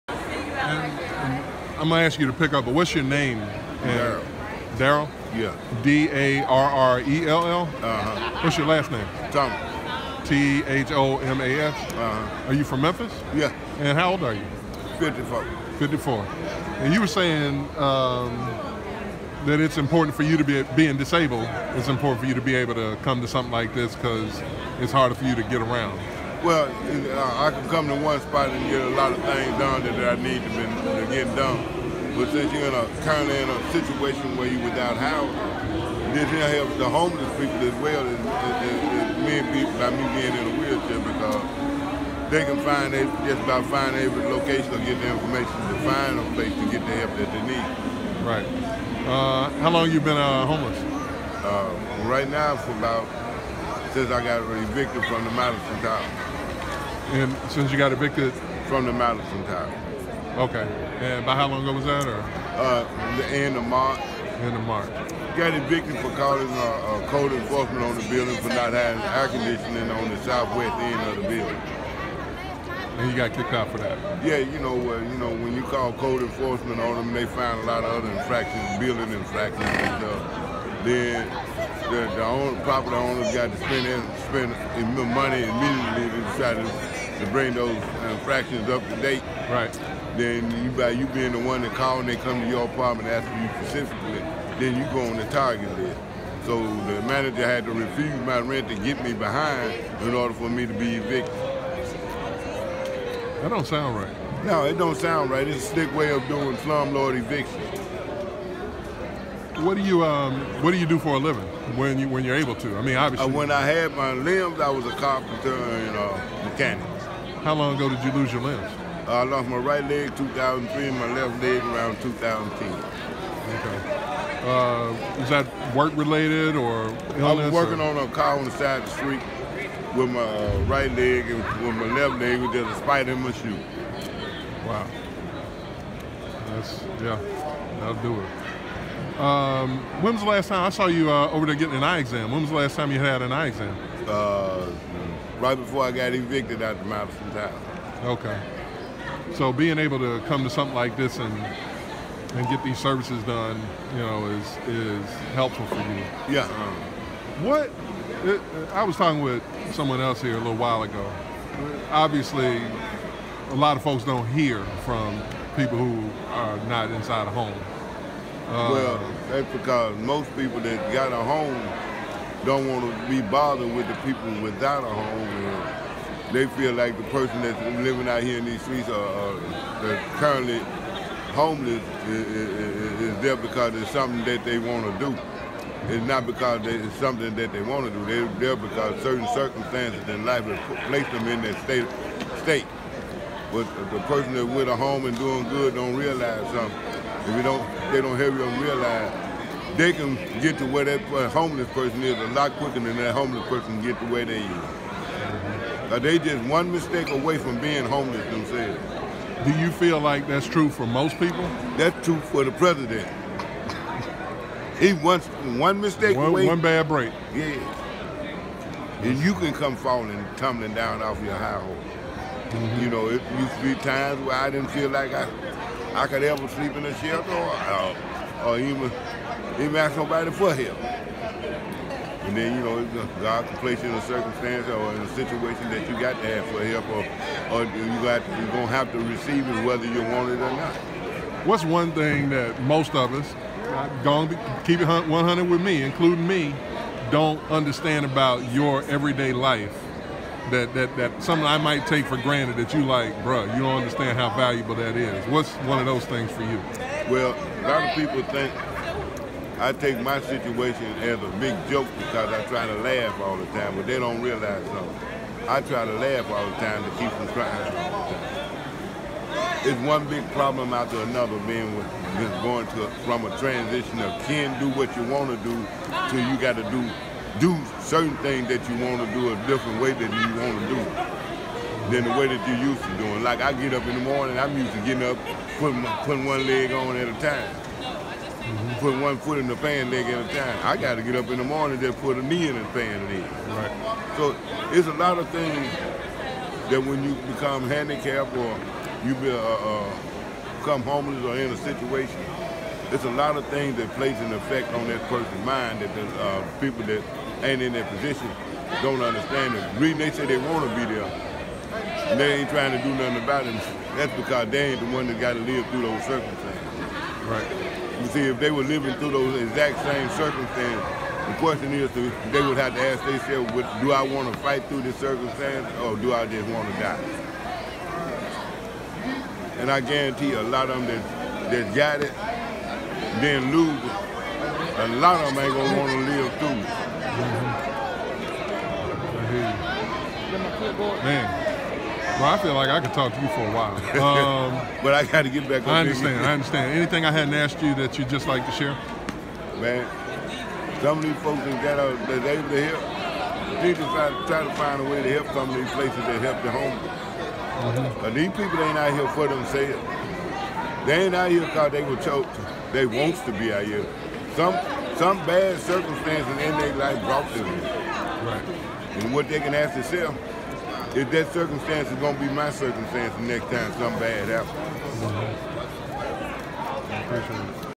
TSDtv: Homeless in Memphis: Darrell Thomas, 54
"A lot of people think I take my situation as a big joke, because I try to laugh all the time. But they don’t realize: I try to laugh all the time to keep from crying all the time.”